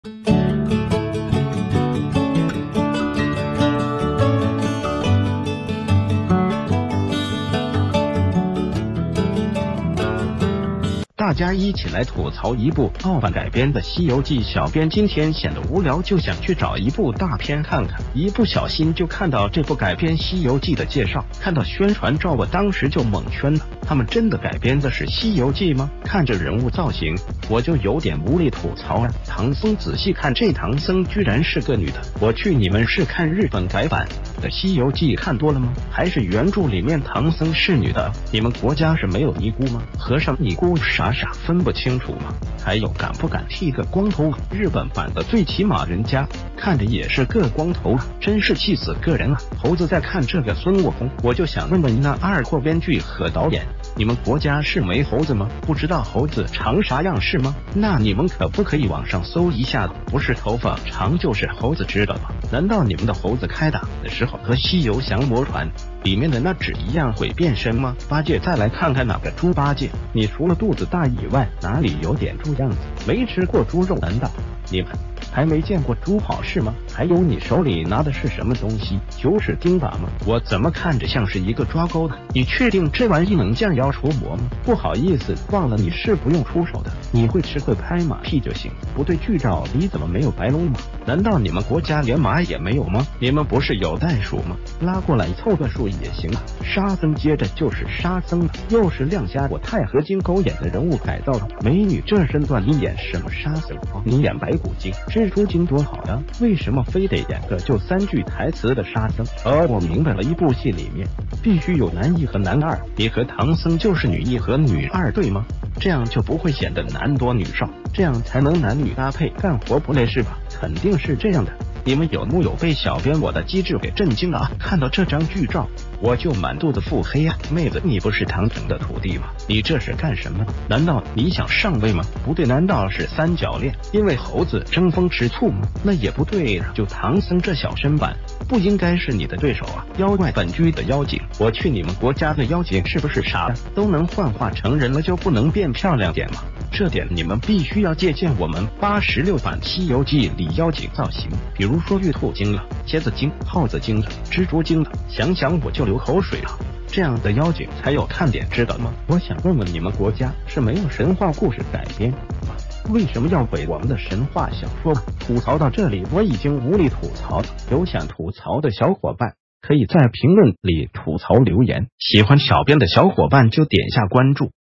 澳洲版改編的《西游記》你看過嗎？沒有就聽我給你們吐槽一下
大家一起來吐槽一部澳版改編的《西游記》
    小編今天顯得無聊就想去找一部大片看看，一不小心就看到這部改編西游記的介紹。看到宣傳照我當時就懵圈了，他們真的改編的是西游記嗎，看這人物造型我就有點無力吐槽啊。
    唐僧
    仔細看這唐僧居然是個女的，我去你們是看日本改版的西游記看多了嗎？還是原著里面唐僧是女的啊？你們國家是沒有尼姑嗎？和尚尼姑傻傻分不清楚嗎？還有敢不敢剃個光頭啊，日本版的最起碼人家看著也是個光頭啊，真是氣死個人啊。
    猴子
    再看這個孫悟空，我就想問問那二貨編劇和導演你們國家是沒猴子嗎，不知道猴子長啥樣是嗎，那你們可不可以網上搜一下啊，不是頭發長就是猴子知道嗎。難道你們的猴子開打的時候和《西游降魔傳》里面的那只一樣會變身嗎？
    八戒
    再來看看那個豬八戒，你除了肚子大以外哪里有點豬樣子，沒吃過豬肉難道你們還沒見過豬跑是嗎。還有你手里拿的是什么東西，九尺釘鈀嗎？我怎么看著像是一個抓鉤啊你確定這玩意能降妖除魔嗎？不好意思忘了你是不用出手的，你會吃會拍馬屁就行，不對劇照里怎么沒有白龍馬，難道你們國家連馬也沒有嗎？你們不是有袋鼠嗎，拉過來湊個數也行啊。
    沙僧
    接著就是沙僧了，又是亮瞎我鈦合金狗眼的人物改造啊。美女這身段你演什么沙僧啊你演白骨精，蜘蛛精多好啊，為什么非得演個就三句臺詞的沙僧。哦我明白了一部戲里面必須有男一和男二，你和唐僧就是女一和女二對嗎?這樣就不會顯得男多女少，這樣才能男女搭配干活不累是吧。肯定是這樣的，你們有木有被小編我的機智給震驚了啊。
    看到這張劇照我就滿肚子腹黑啊，妹子你不是唐僧的徒弟嗎？你這是干什么啊？難道你想上位嗎？不對難道是三角戀因為猴子爭風吃醋嗎，那也不對啊就唐僧這小身板不應該是你的對手啊。
    妖怪
    本劇的妖精，我去你們國家的妖精是不是傻啊？都能幻化成人了就不能變漂亮點嗎，這點你們必須要借鑒我們86版西游記里妖精造型，比如說玉兔精了， 蝎子精， 耗子精了，蜘蛛精了。想想我就留口水啊這樣的妖精才有看點知道嗎。我想問問你們國家是沒有神話故事改編嗎為什么要毀我們的神話小說啊。吐槽到這里我已經無力吐槽了，有想吐槽的小伙伴可以在評論里吐槽留言，喜歡小編的小伙伴就點下關注。原創作品，盜者必究。